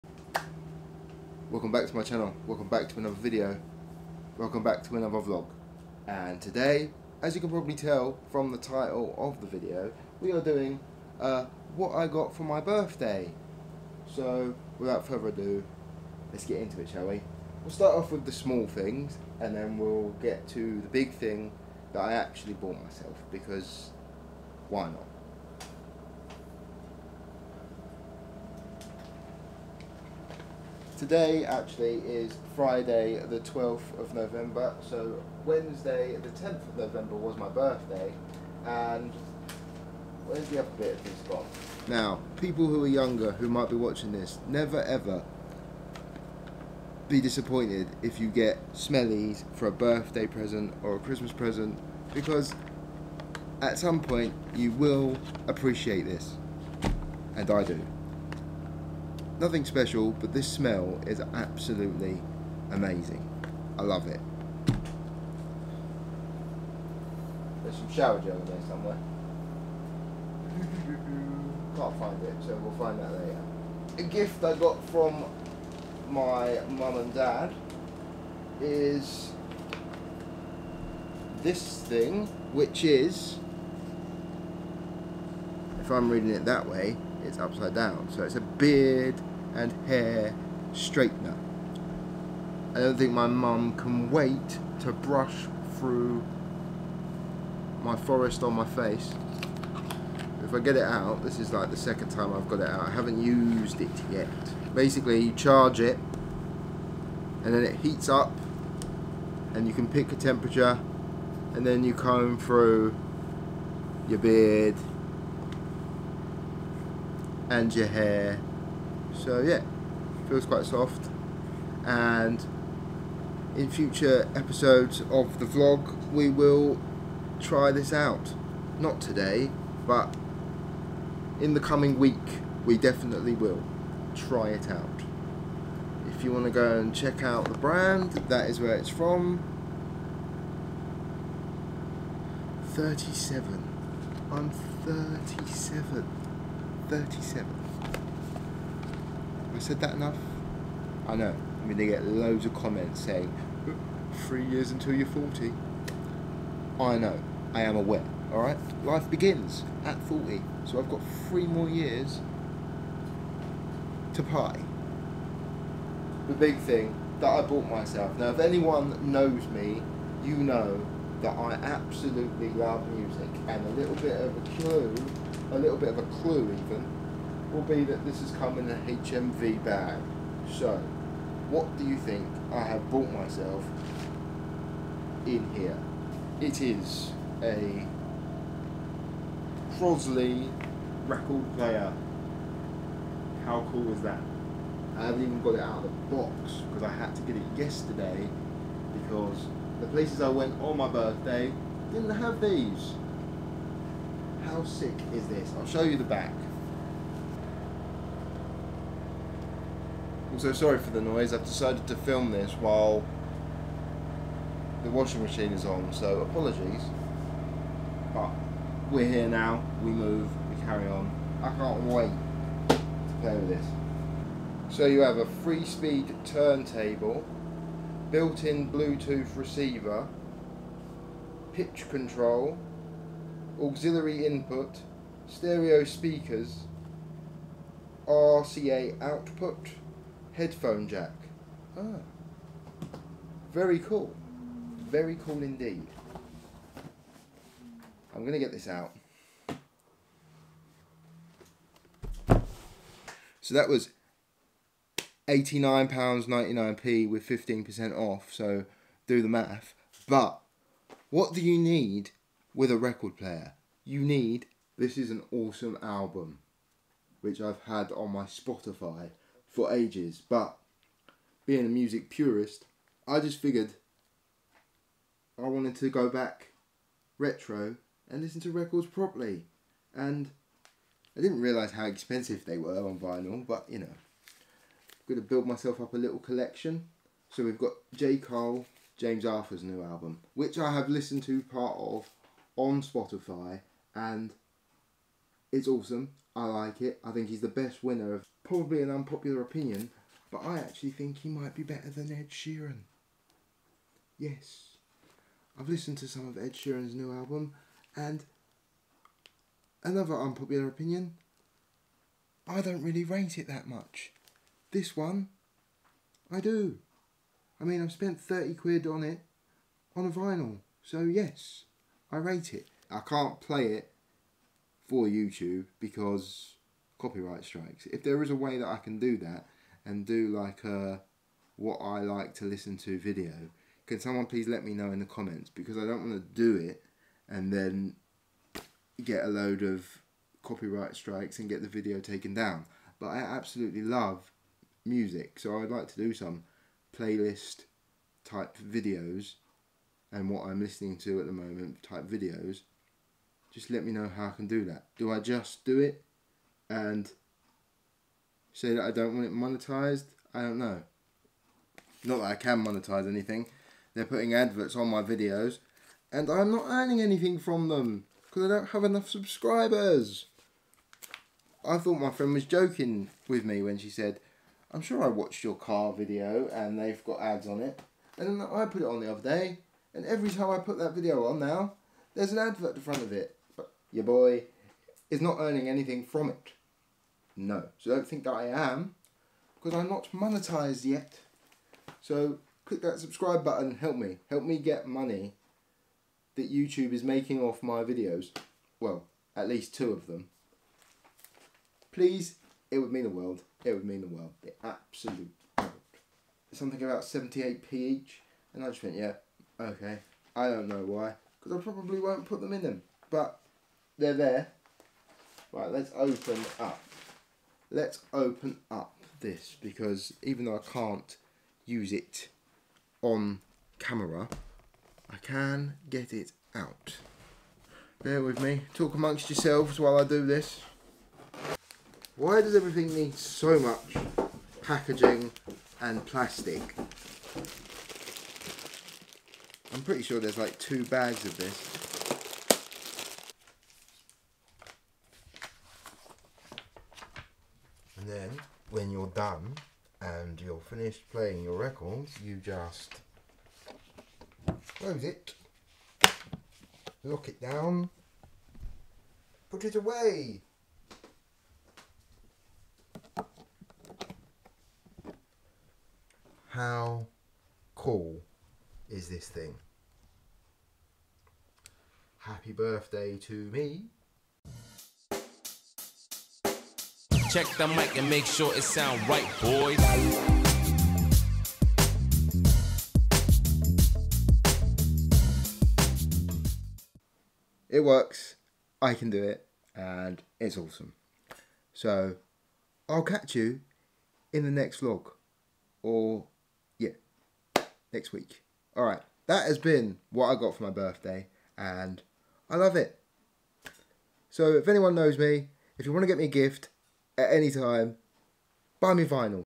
HAPPY BIRTHDAY TO ME
It was my birthday and this is what I got. Doing that YouTuber thing! Enjoy LJx